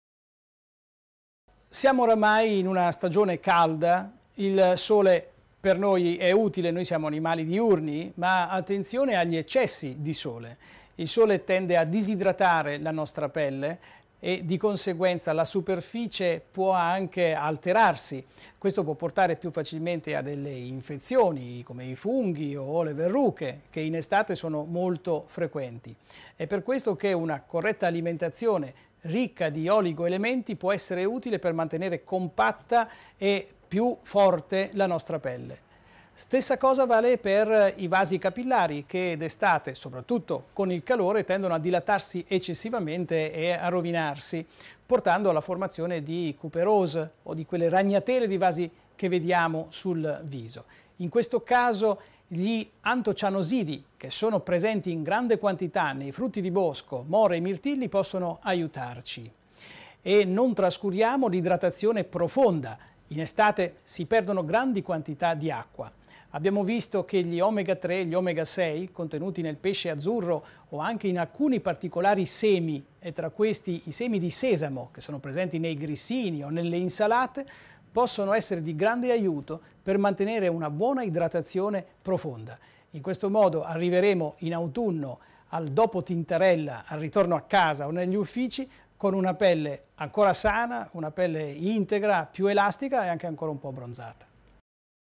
La pelle al sole - Dermatologo Prof. Antonino Di Pietro
Il dermatologo Prof. Antonino Di Pietro, dermatologo plastico e presidente ISPLAD, in questa intervista ci spiega come affrontare i problemi derivanti dall'esposizione al sole e dal calore.